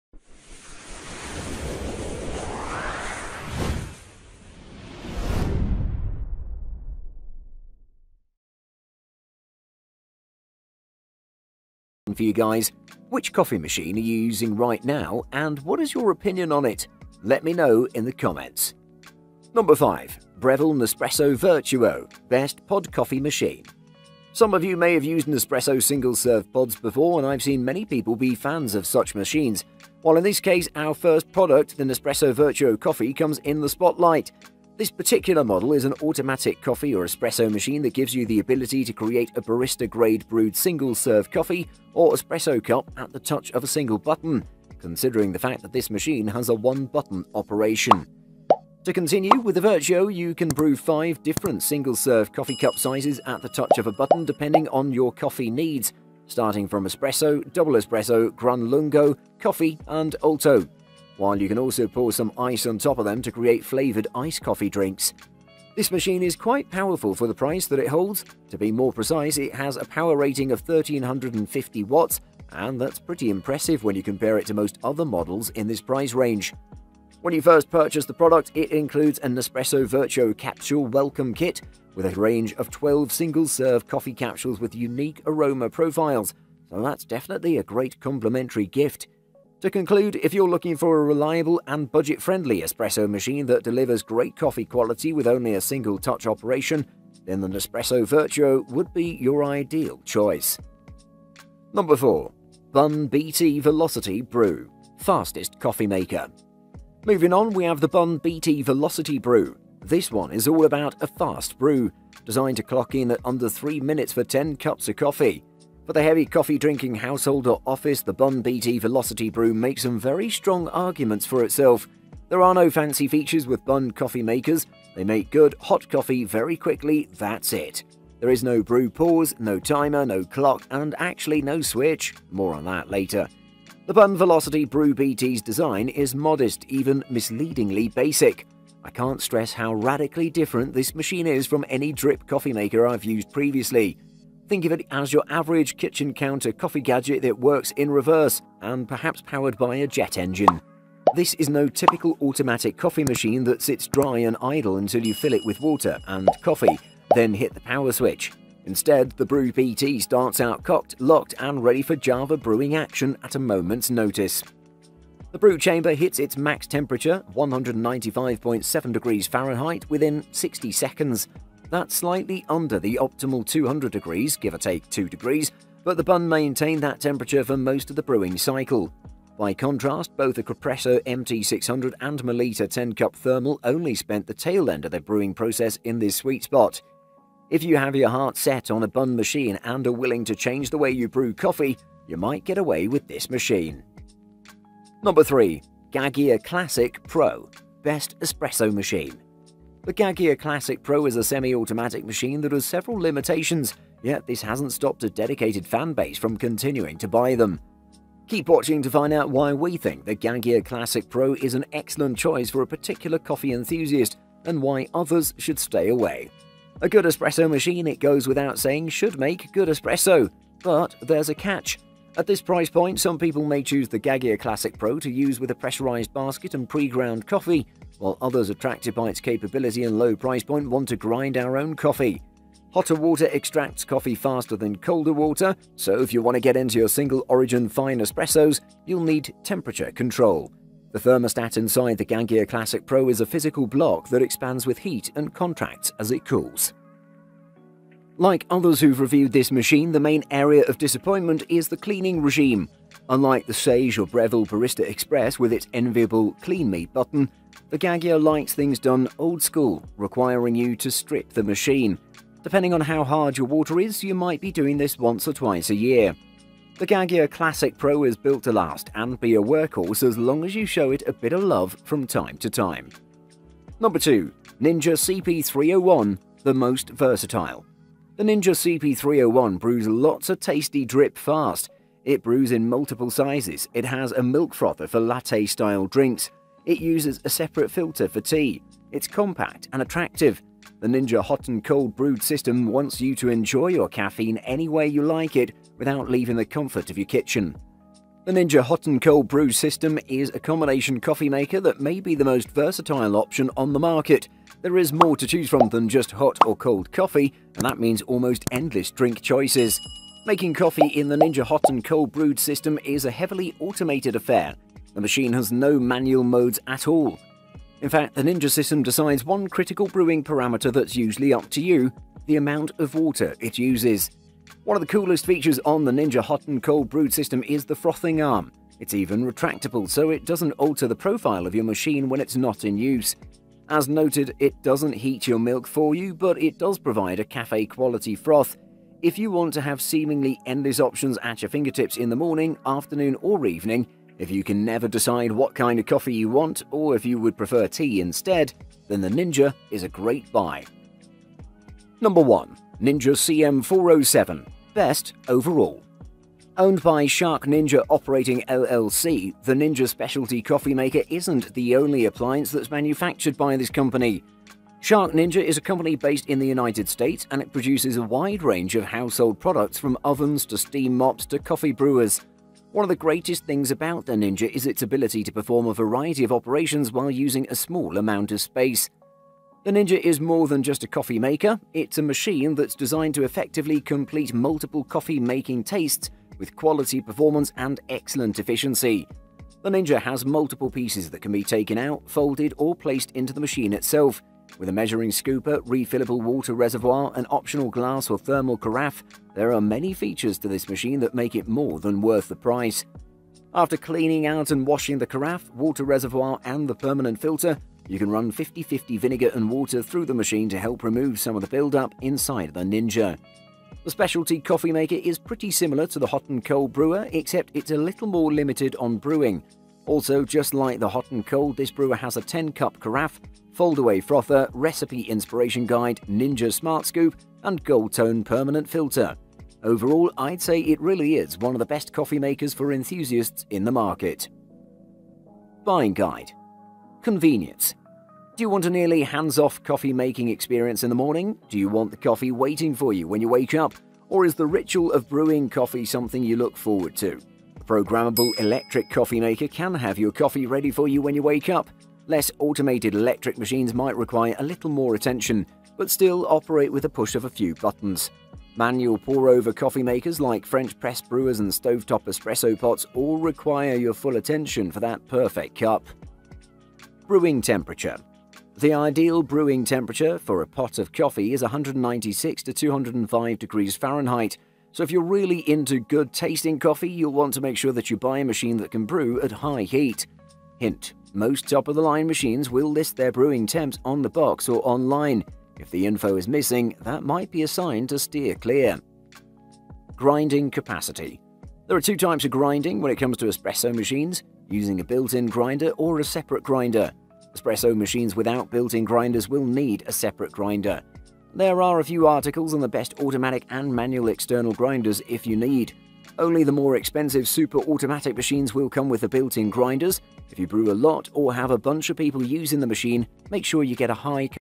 ✅ 2021 Review Gaggia RI9380/46 Classic Pro | Top 5 Best Coffee Machines in 2021
► Links to the Best Coffee Machines 2021 we listed in this video:

► 5 - Breville/Nespresso Vertuo
► 4 - BUNN BT Velocity Brew
► 3 - Gaggia Classic Pro
► 2 - Ninja CP301
► 1 - Ninja CM407